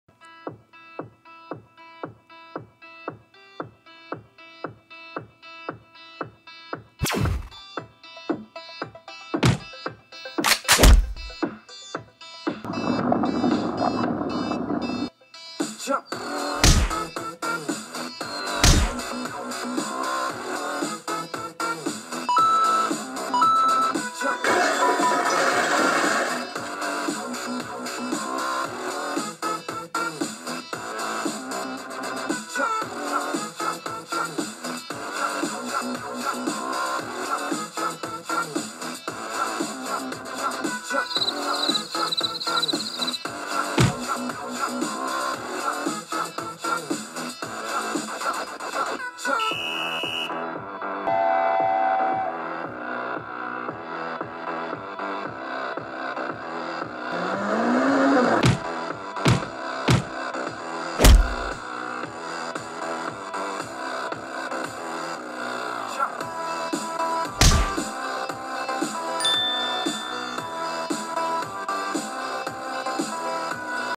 Toy Brawl 2
Sequel to "Toy Brawl". Please tell me if you enjoyed this and what you want to see next.